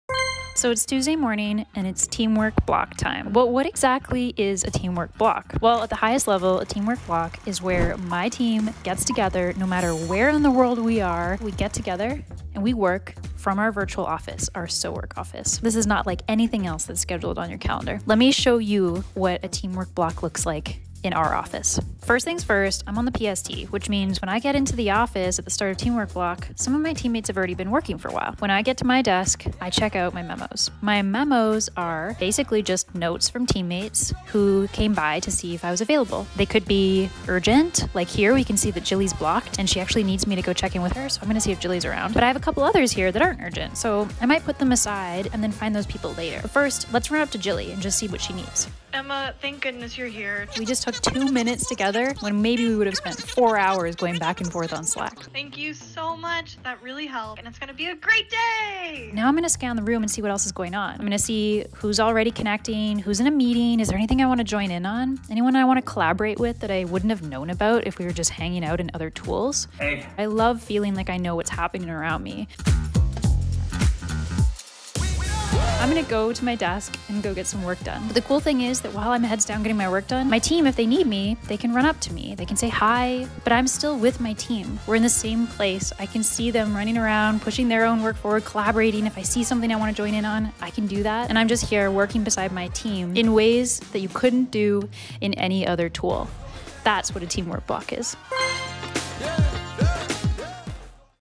Setting Up 'Teamwork Blocks' in Your SoWork Virtual Office
Learn how teams at SoWork use teamwork blocks to communicate and move work forward.